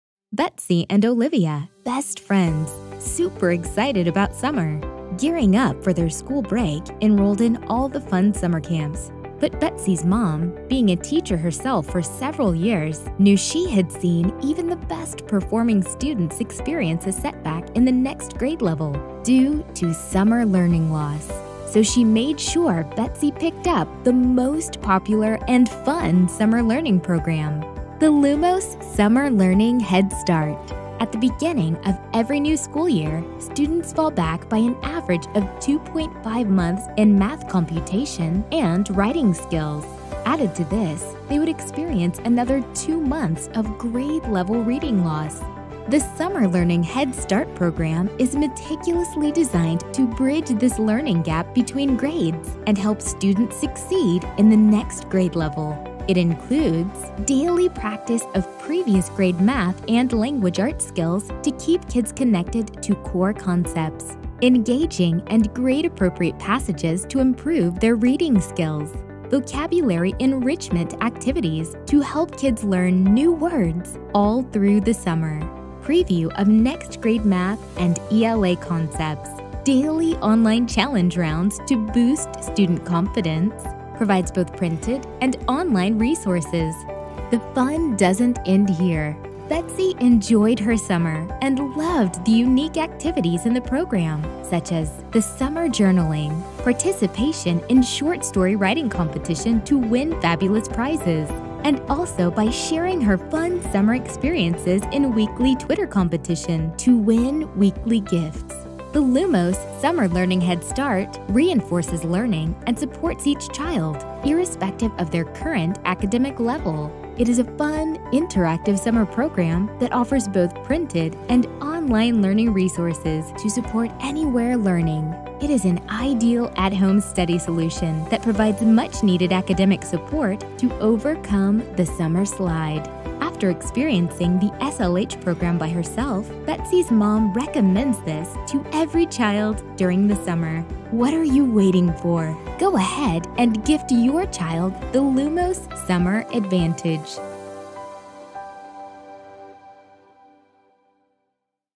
Help Kids Beat Summer Slide with Fun & Interactive Summer Program | Grade 2-10 | Print & Online Mode
In this video, you will learn how to take advantage of the Lumos Summer Learning HeadStart™ Program to help your child beat the summer slide.

The Summer Learning HeadStart™ is a fun, interactive summer program that offers both printed and online learning resources to support anywhere learning. It is an ideal at-home study solution that provides much-needed academic support to overcome the summer slide.

It is meticulously designed by expert teachers to help students from grade-2 to grade-10 retain and strengthen their Math and English Language Arts skills during the summer months. It includes:

-Daily Math and English Language Arts Practice
-Practice for Previous Grade & Next Grade
-Engaging Reading activities
-Vocabulary Enrichment Activities
-Free Entry to Summer Short Story Competition and weekly Twitter Competition